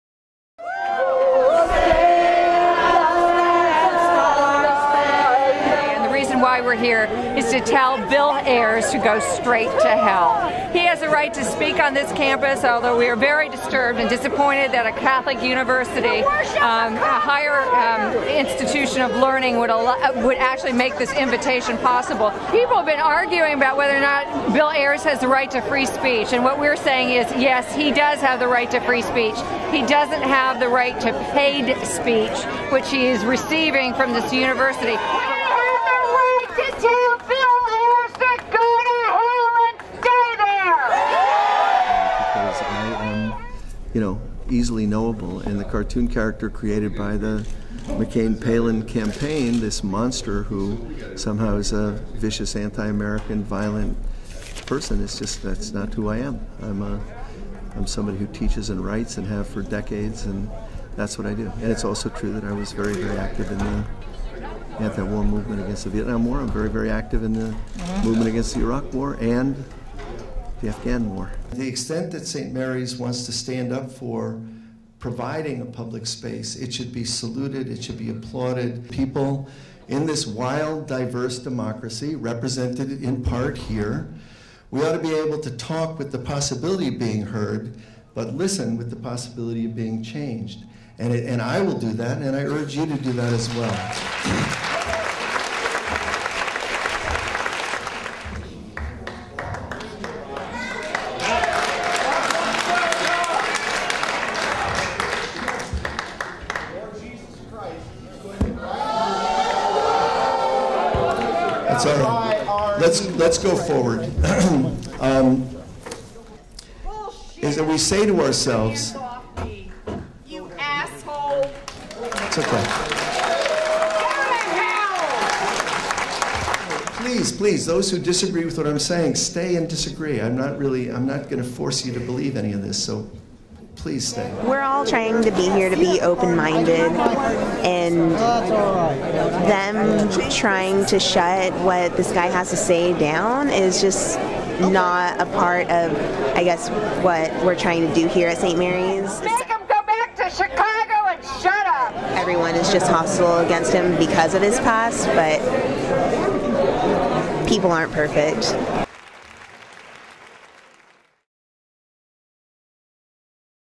Bill Ayers Speaks at Saint Mary's College
Former Weather Underground leader and current professor Bill Ayers speaks at Saint Mary's College in Moraga as conservatives protest.